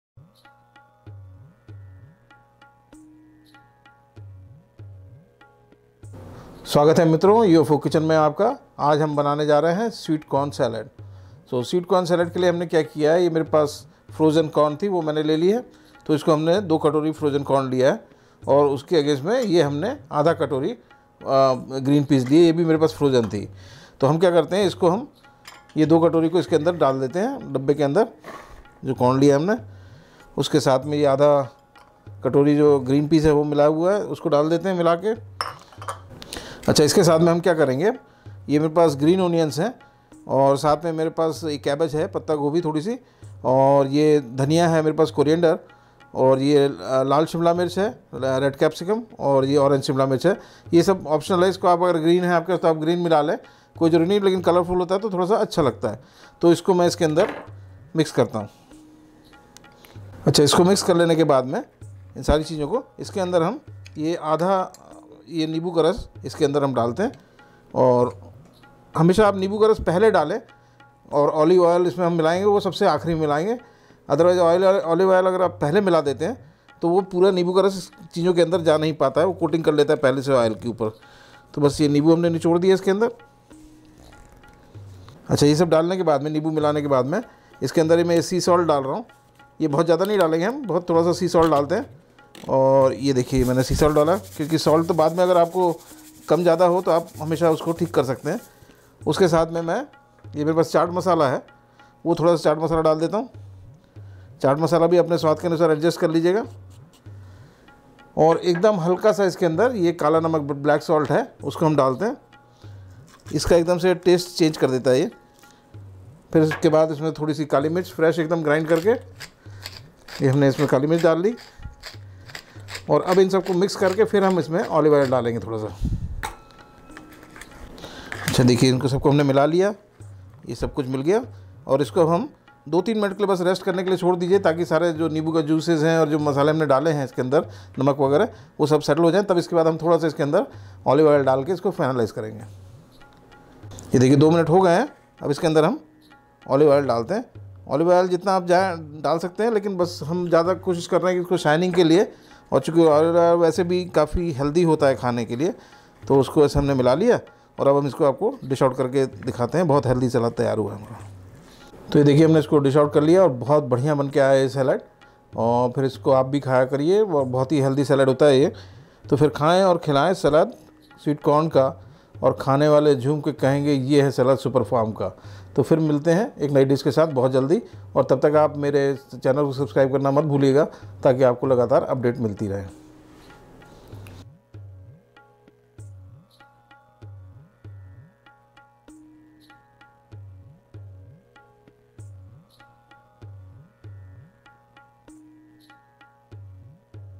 कॉर्न सलाद घर में बनाओं और मजे से खाओ - Sweet Corn Salad
Today we are making healthy and yummy sweet corn salad:

Ingredients:
कार्न (भुट्टे का दाना)- Sweet Corn – 2 Cup
हरी मटर – Green Peas  –1/2 cup
हरी प्याज - Green Onion – 2/3 Stems
पत्ता गोभी – Cabbage – ¼
धनिया पत्ती – Coriander – Small Bunch
लाल शिमला मिर्च – Red Capcicum – 1/4
ऑरेन्ज शिमला मिर्च – Orange Capcicum – ¼
नींबू – Lemon Juice – ½
नमक – Salt- to Taste – small pinch
चाट मसाला -  Chat Masala – ½ Tea Spoon
काली मिर्च – Black Pepper -  ¼ Tea Spoon
काला नमक – Black Salt (Rock Salt) – ¼ Tea Spoon
ऑलिव आयल – Olive Oil – ½ Tea Spoon